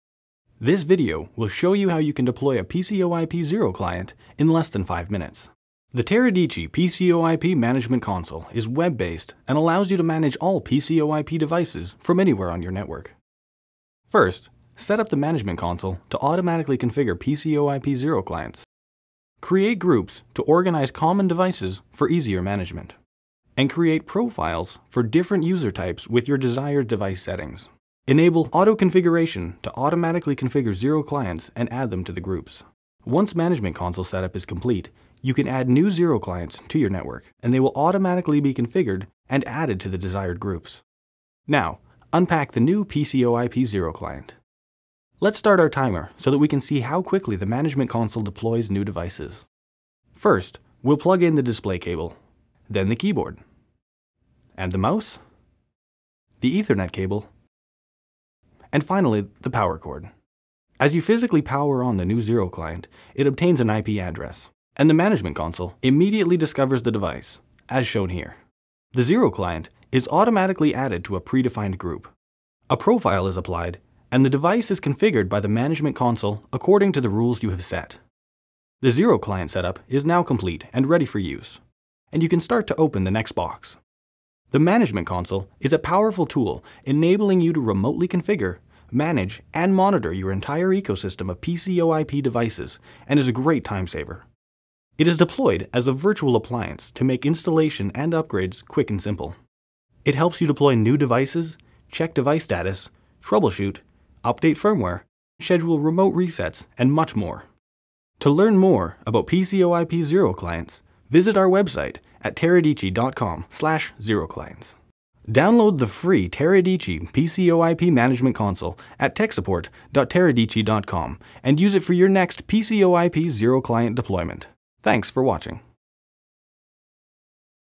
How to set up a PCoIP zero client in 5 minutes - a step by step guide
This video provides a step by step guide to setting up a PCoIP zero client - watch this video to learn how to set up and configure PCoIP zero client in less than 5 minutes.